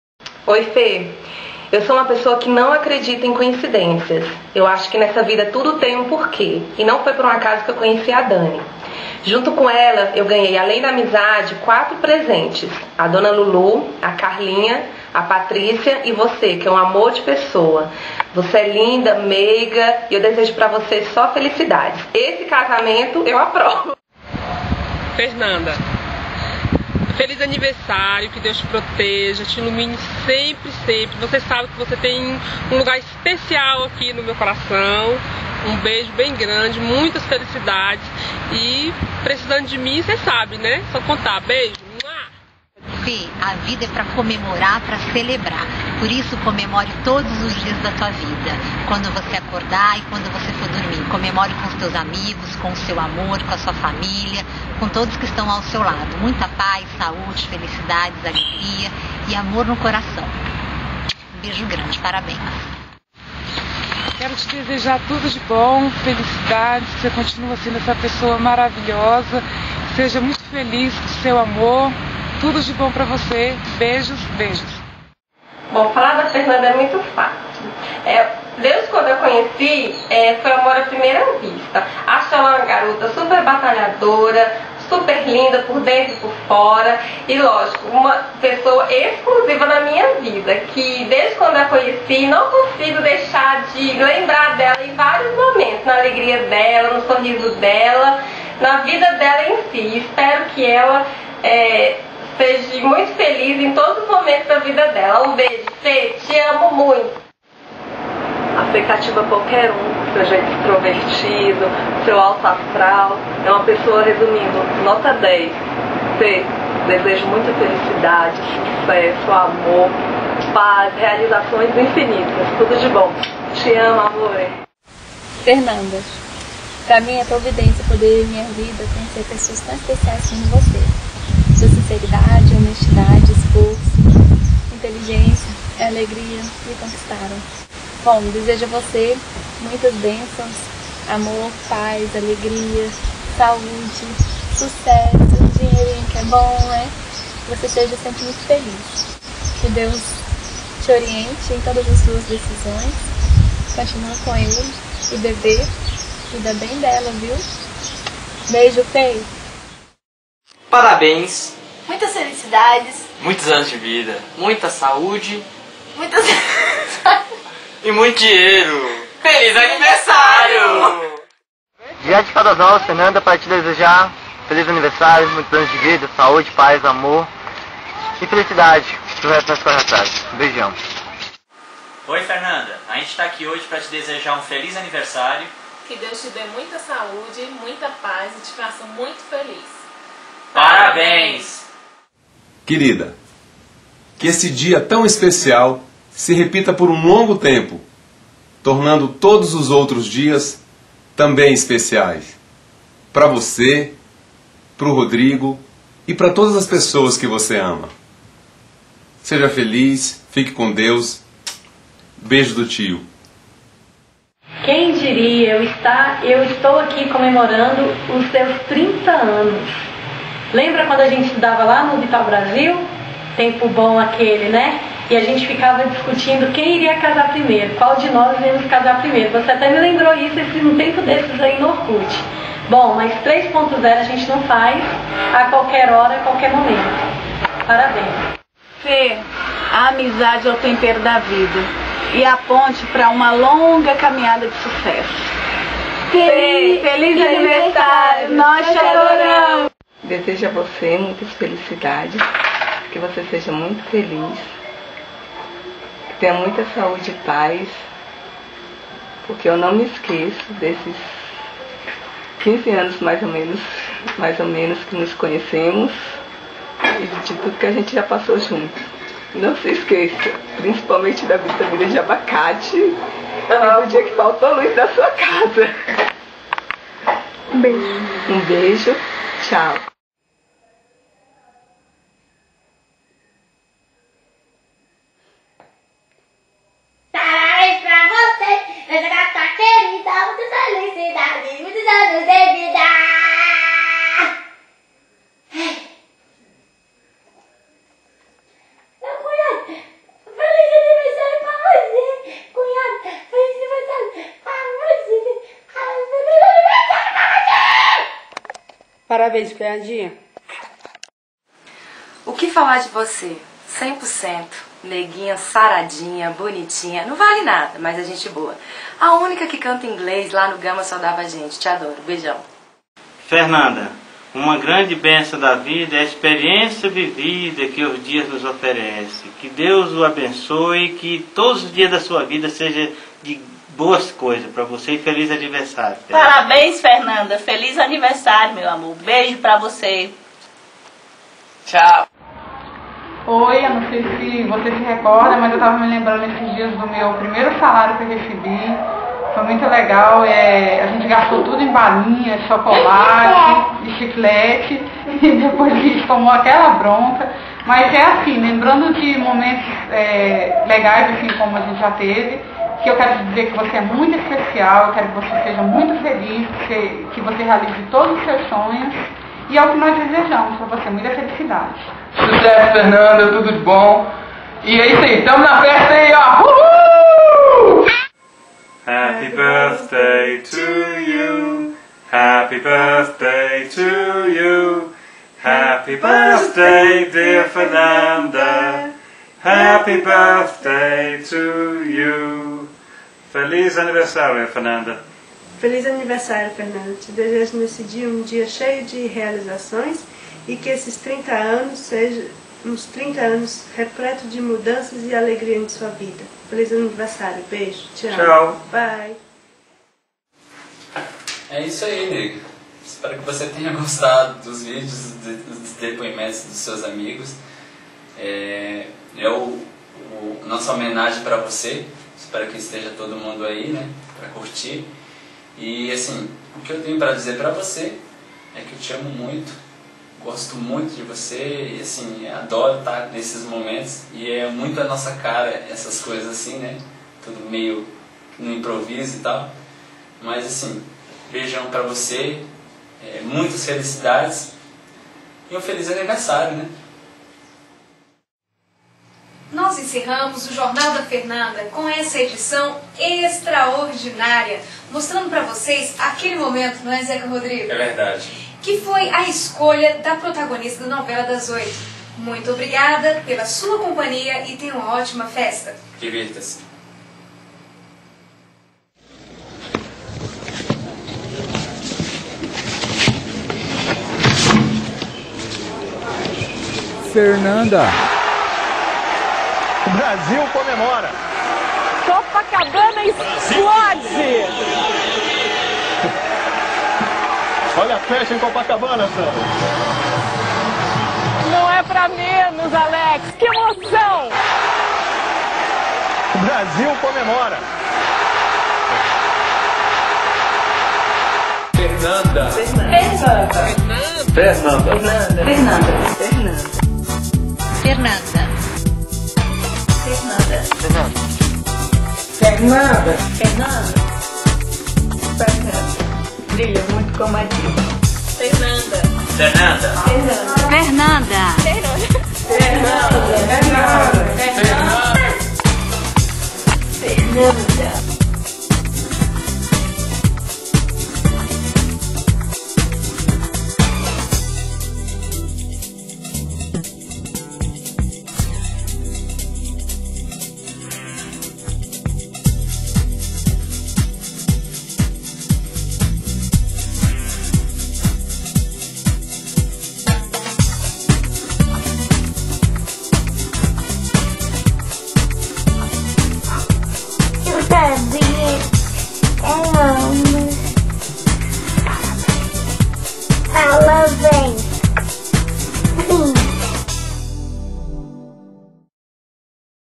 Jornal da Fernanda 2° Parte
Uma divertida homenagem feita para Fernada por seus amigos e familiares no dia do seu aniversário de 30 anos.